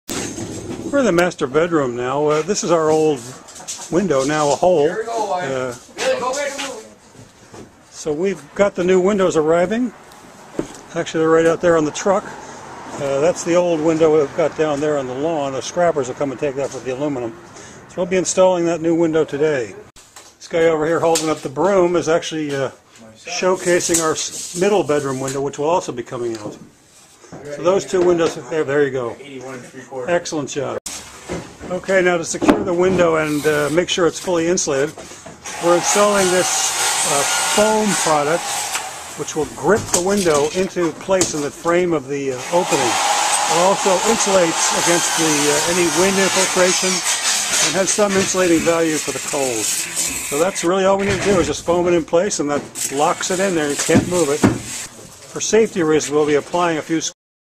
Reno Series 2.36-New Bedroom Windows
This is the 36th episode of our second video series on renovation / new building. We'll show you how these houses were prepared for resale.

Most of our windows are new and we`ll keep them. The master and middle bedrooms get new ones, which are installed using a specialty foam and screws.